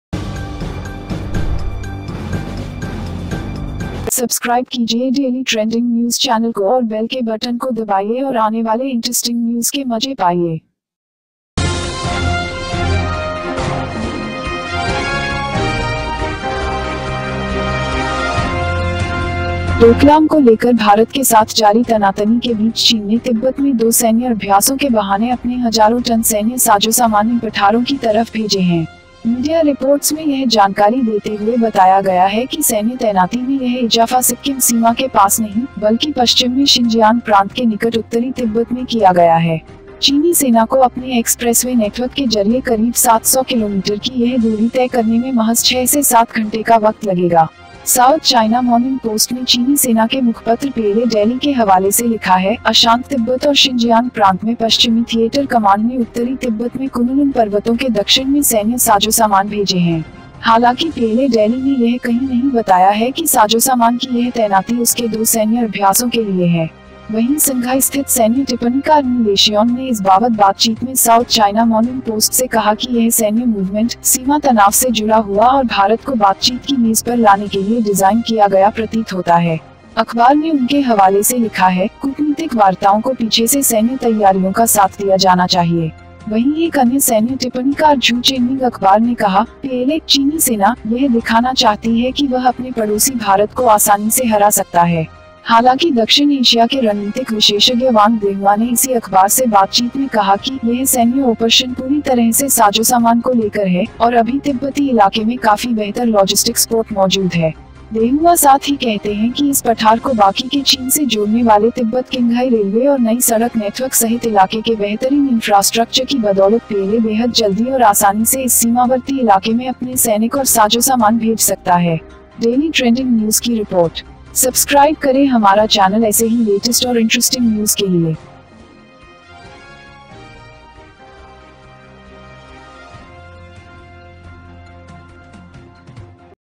चीन ने हजारों टन सैन्य साजोसामान तिब्बत भेजा, क्या भारत से युद्ध की है तैयारी ??
डोकलाम को लेकर भारत के साथ जारी तनातनी के बीच चीन ने तिब्बत में दो सैन्य अभ्यासों के बहाने अपने हजारों टन सैन्य साजोसामान इन पठारों की तरफ भेजे हैं.